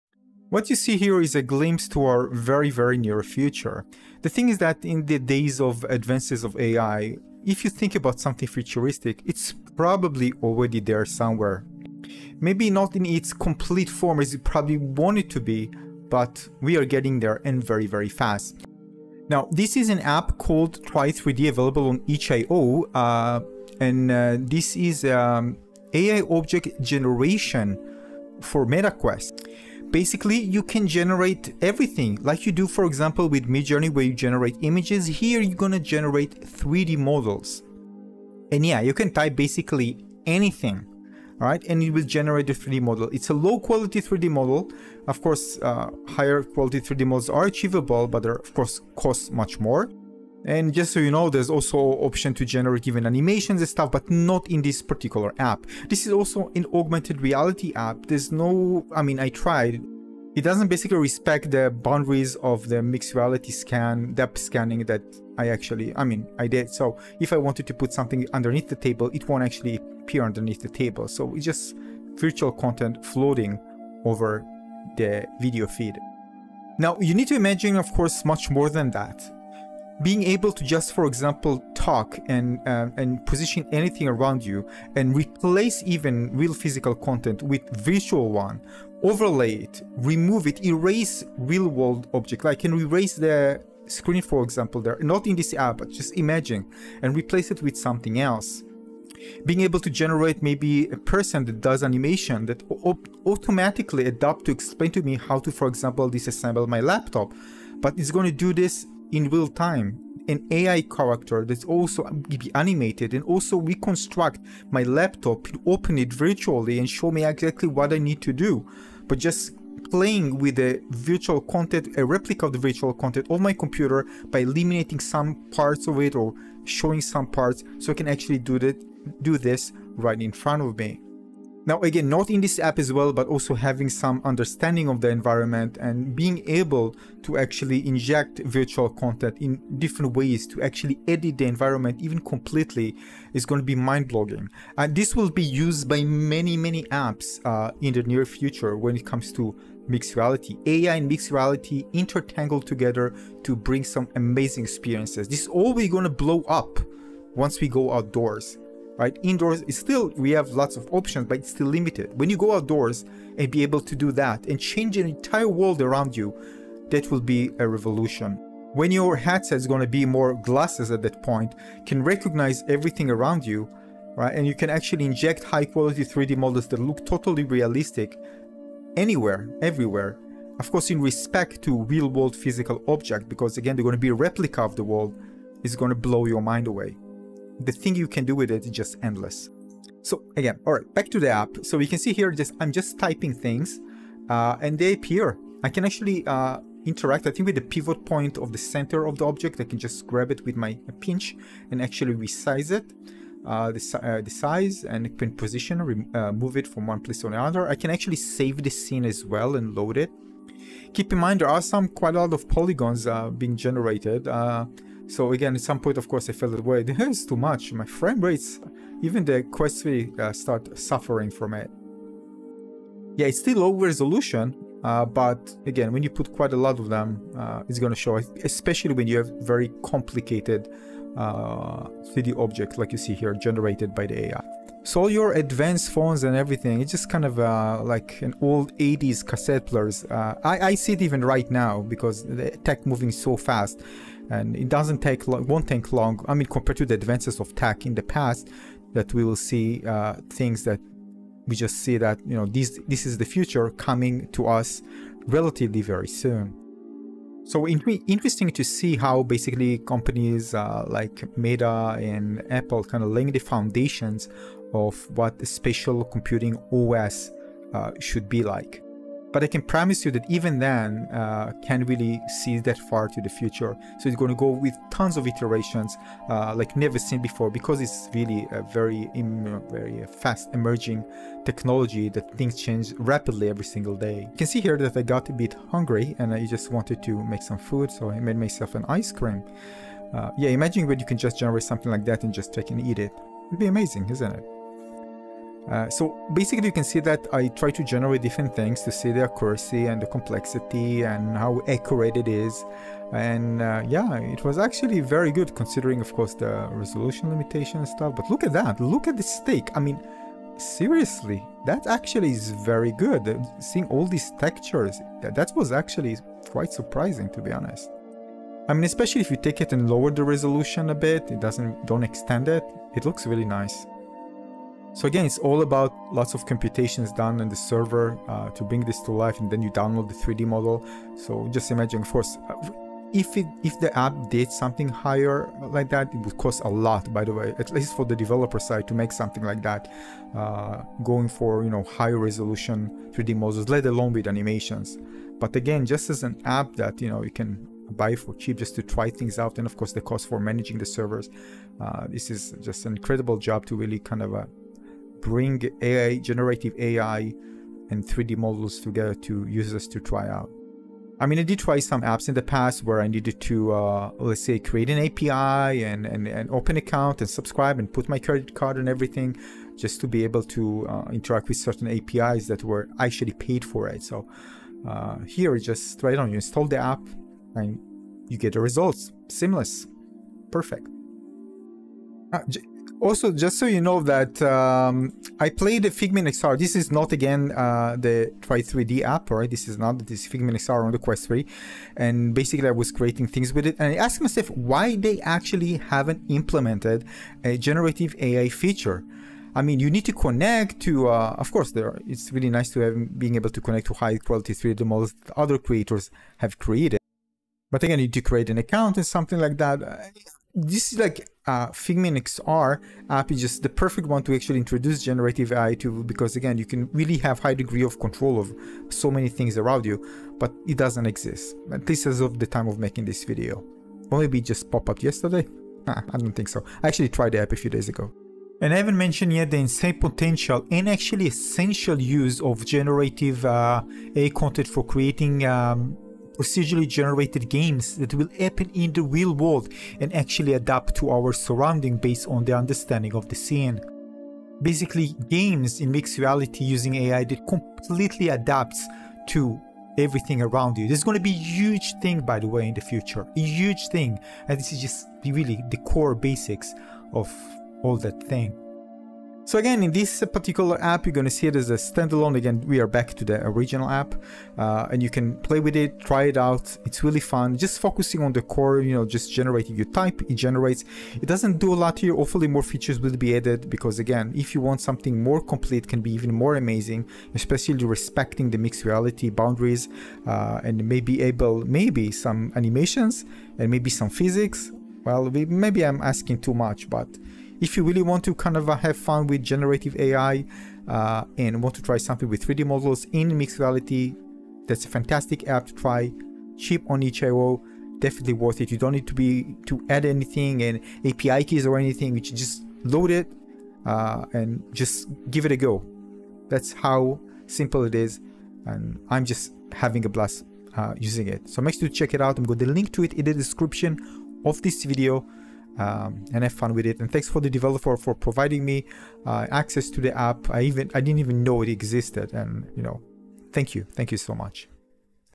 Use AI to Generate 3D Models in Mixed Reality by Typing Text (Text-to-3D) | Meta Quest
Imagine Midjourney like Generative AI but for 3D model generation using text prompt and in mixed reality for Meta Quest 2/Quest 3 VR headset? This is Tri3D (or TriVR), an artificial intelligence (AI) mixed reality app developed by cazforshort.  Use a prompt to write text to describe the 3D model you want to generate and interact with it in MR.  Text-based 3D modeling, a Text-to-3D model generation using AI for an AI-powered 3D design.